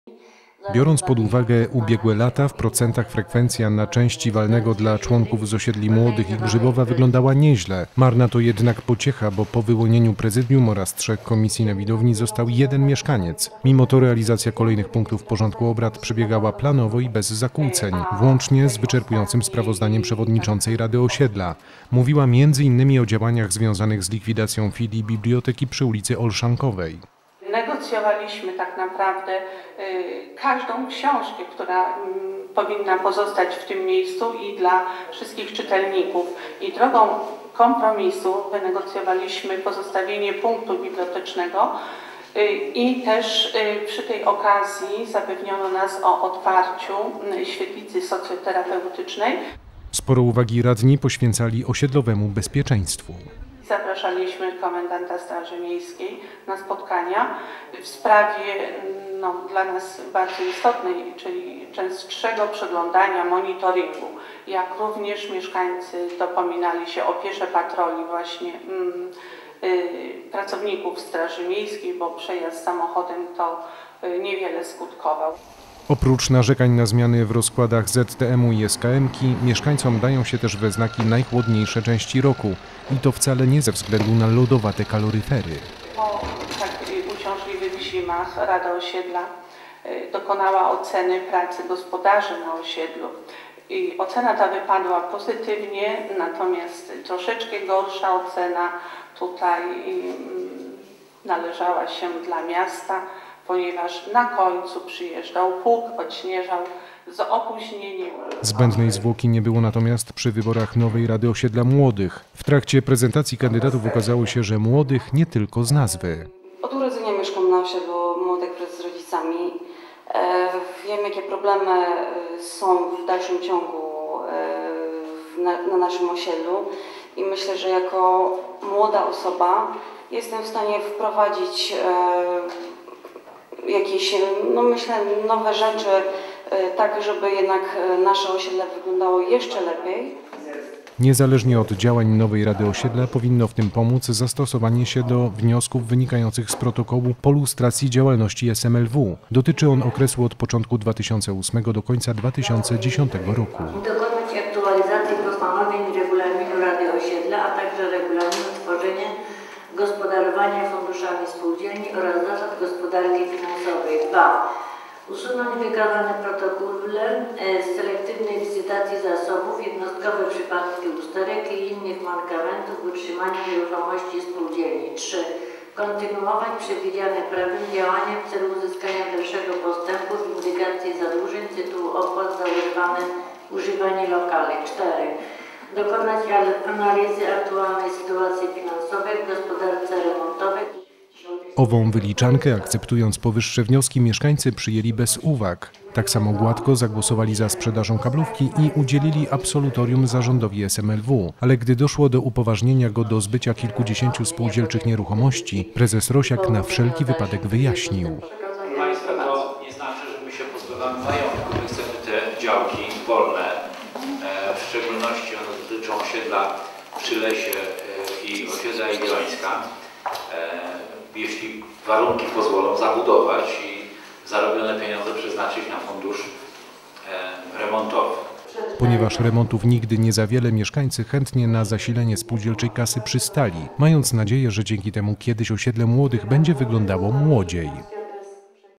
Walne Sobieskiego NS
We wtorek na piątej grupie walnego zgromadzenia członków SML-W spotkali się
mieszkańcy osiedla Sobieskiego. Podobnie jak i na innych zebraniach frekwencja nie była
imponująca. Na 2226 uprawnionych do głosowania członków, na sali było obecnych 71.